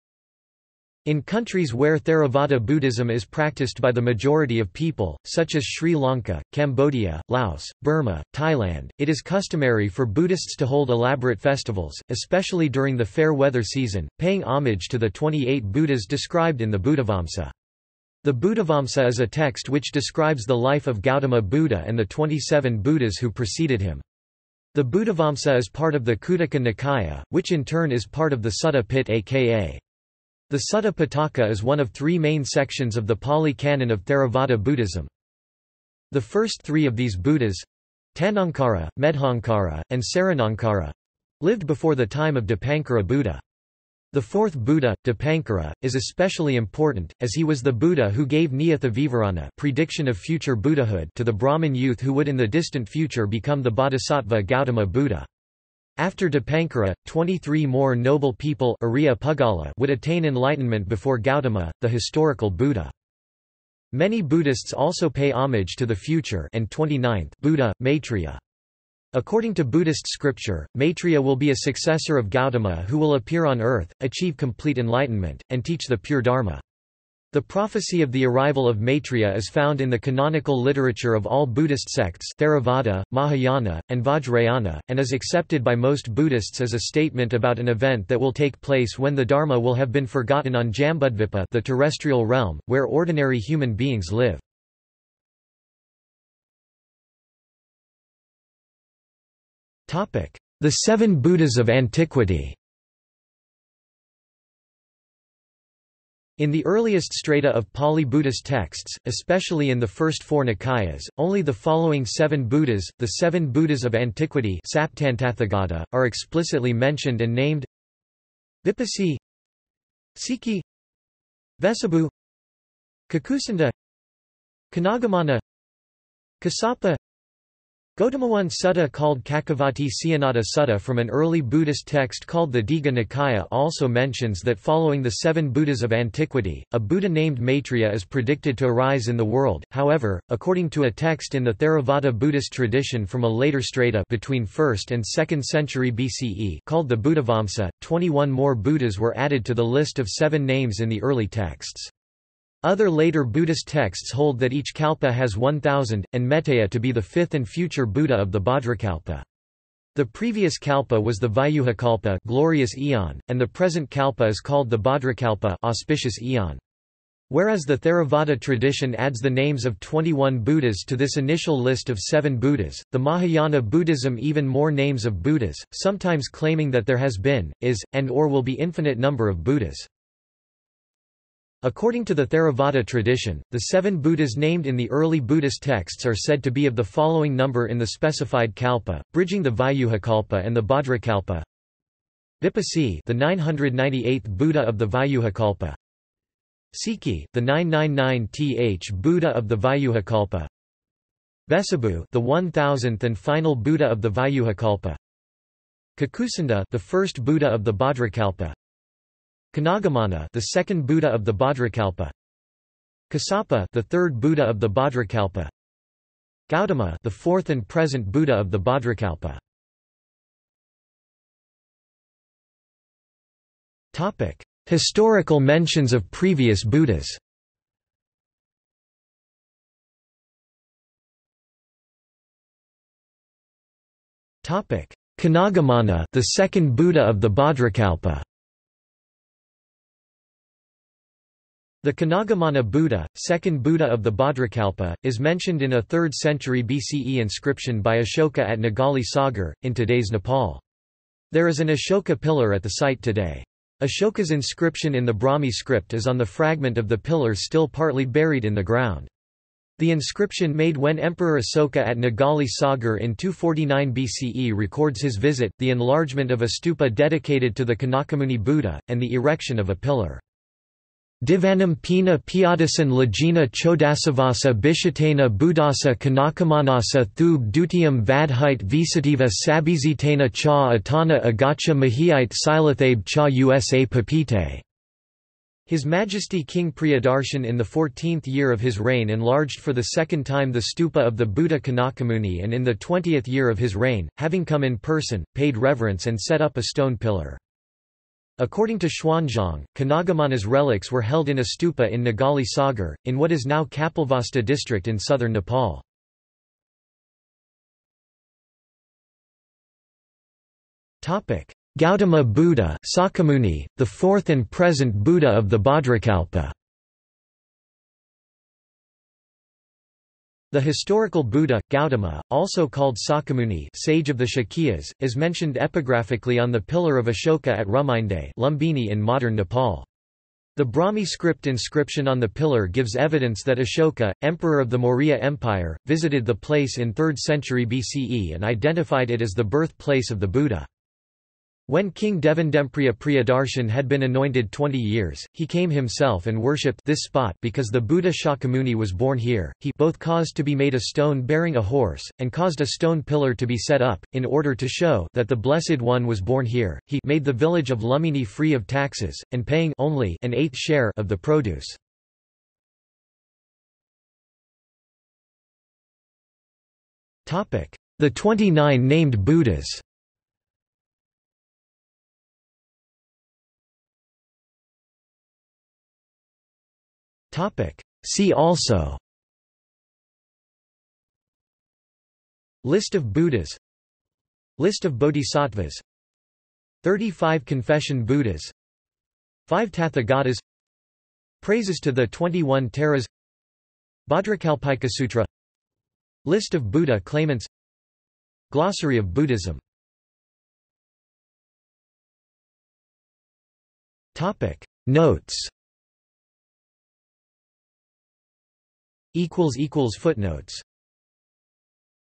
List of the named Buddhas | Wikipedia audio article
This is an audio version of the Wikipedia Article:
List of the named Buddhas

00:01:36 1 The Seven Buddhas of Antiquity
00:03:40 2 Historical mentions of previous Buddhas
00:03:51 2.1 Koṇāgamana (the second Buddha of the bhadrakalpa)
00:05:07 2.2 Gautama Buddha (Sakyamuni, the fourth and present Buddha of the bhadrakalpa)
00:06:03 3 The 29 named Buddhas
00:06:13 4 See also
00:06:40 5 Notes

- Socrates

SUMMARY
In countries where Theravāda Buddhism is practiced by the majority of people, such as Sri Lanka, Cambodia, Laos, Burma, Thailand, it is customary for Buddhists to hold elaborate festivals, especially during the fair weather season, paying homage to the 28 Buddhas described in the Buddhavamsa. The Buddhavamsa is a text which describes the life of Gautama Buddha and the 27 Buddhas who preceded him. The Buddhavamsa is part of the Khuddaka Nikāya, which in turn is part of the Sutta Piṭaka. The Sutta Piṭaka is one of three main sections of the Pāli Canon of Theravāda Buddhism.
The first three of these Buddhas—Taṇhaṅkara, Medhaṅkara, and Saraṇaṅkara—lived before the time of Dīpankara Buddha. The fourth Buddha, Dīpankara, is especially important, as he was the Buddha who gave niyatha vivarana (prediction of future Buddhahood) to the Brahmin youth who would in the distant future become the bodhisattva Gautama Buddha. After Dīpankara, 23 more noble people (ariya-puggala) would attain enlightenment before Gautama, the historical Buddha.
Many Buddhists also pay homage to the future (and 29th) Buddha, Maitreya. According to Buddhist scripture, Maitreya will be a successor of Gautama who will appear on Earth, achieve complete enlightenment, and teach the pure Dharma. The prophecy of the arrival of Maitreya is found in the canonical literature of all Buddhist sects (Theravada, Mahayana, and Vajrayana), and is accepted by most Buddhists as a statement about an event that will take place when the Dharma will have been forgotten on Jambudvipa (the terrestrial realm, where ordinary human beings live).